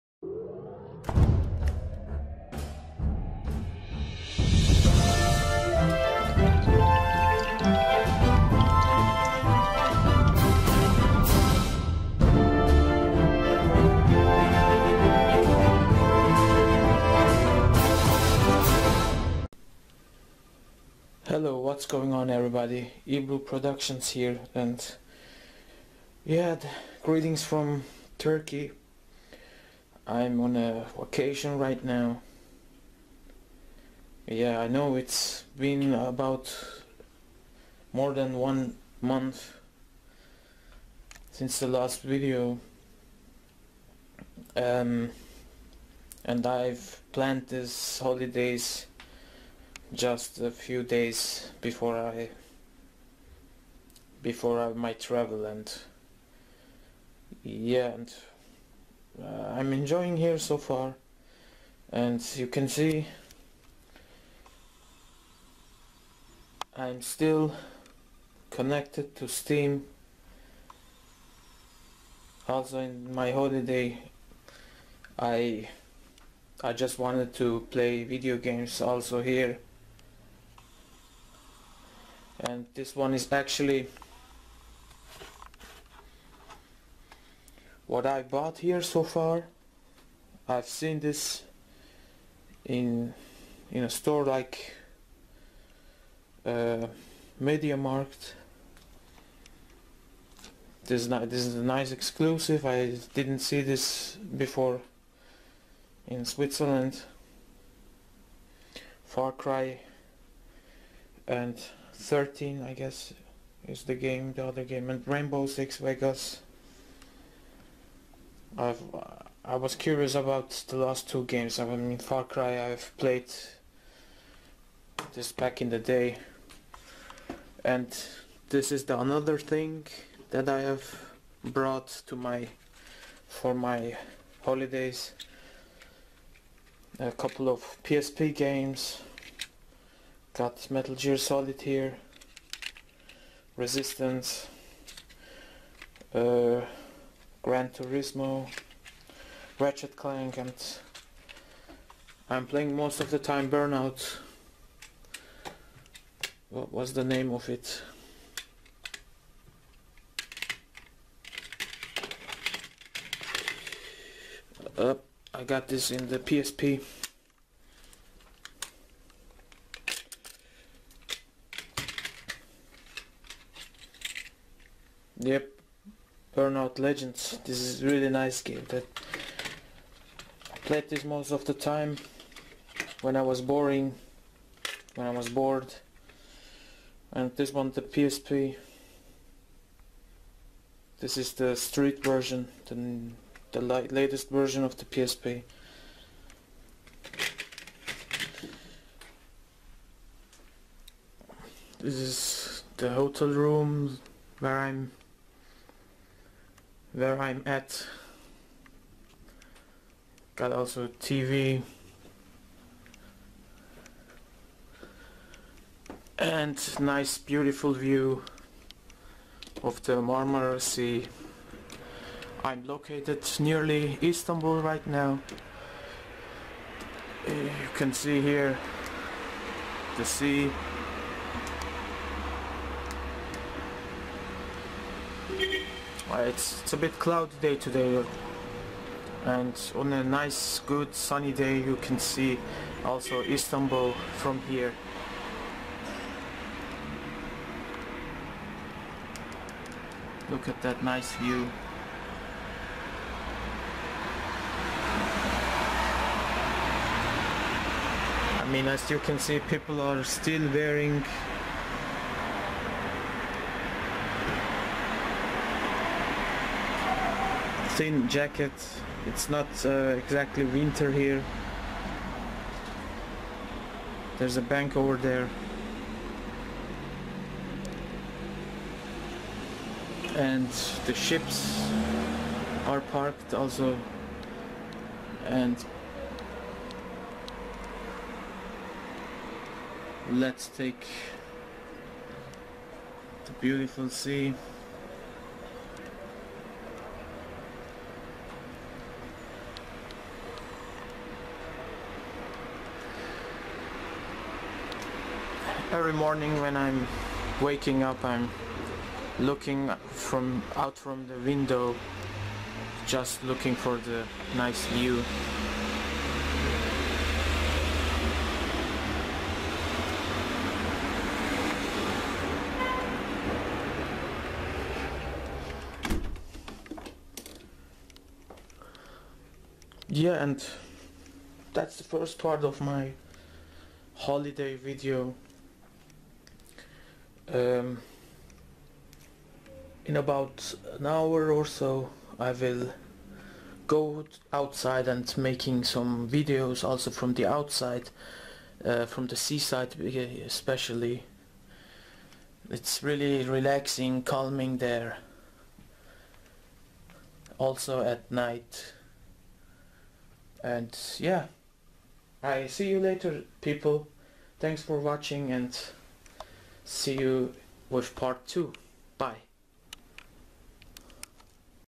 Xmas, New Year 2014: Holidays in Turkey (part1)
Hello all, long time no see. I`m alive and got some video here where i am right now and what i brought for my holidays. Have a nice Xmas and New Year.

Intro music in my video is edited from this song